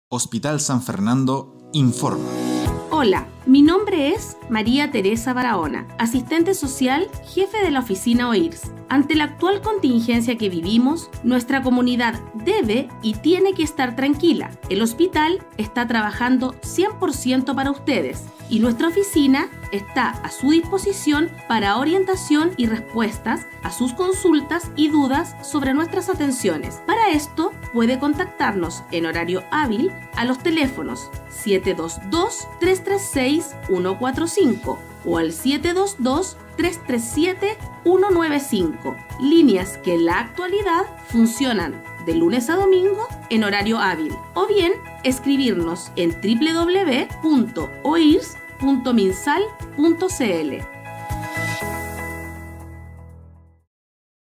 [HSF] Audio-cápsula: Jefa OIRS y el funcionamiento de la oficina durante pandemia
(Audio-cápsula) María Teresa Barahona, asistente social jefa OIRS nos habla sobre el funcionamiento de la Oficina durante pandemia COVID-19.